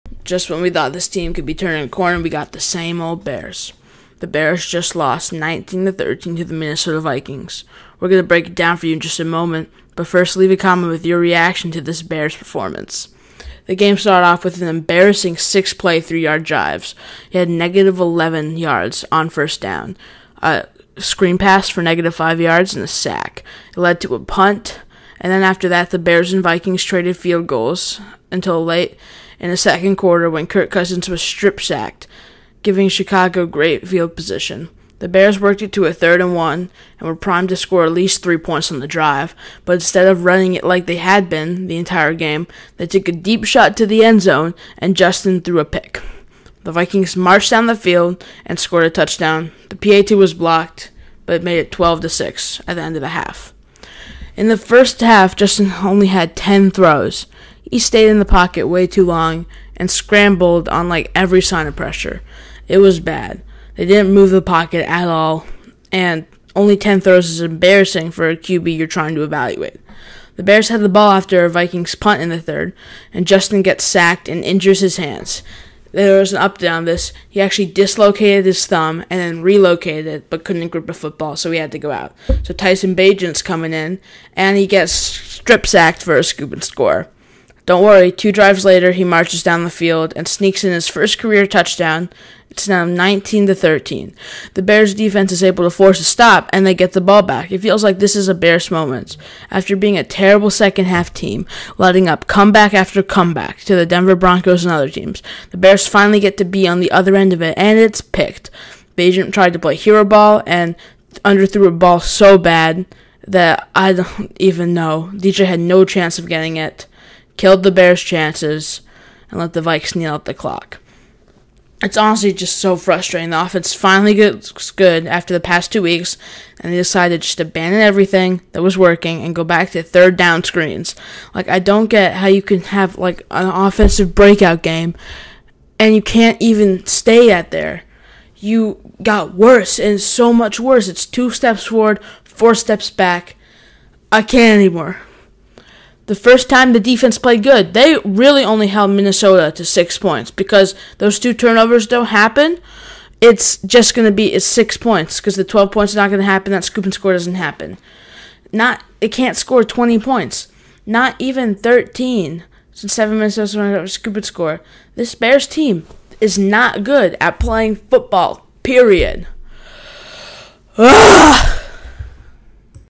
Bears Lose 13-19 to Vikings | Getsy is the worst playcaller in the NFL
The Chicago Bears lost again. Refusing to build on any momentum they had, they kept playing braindead football.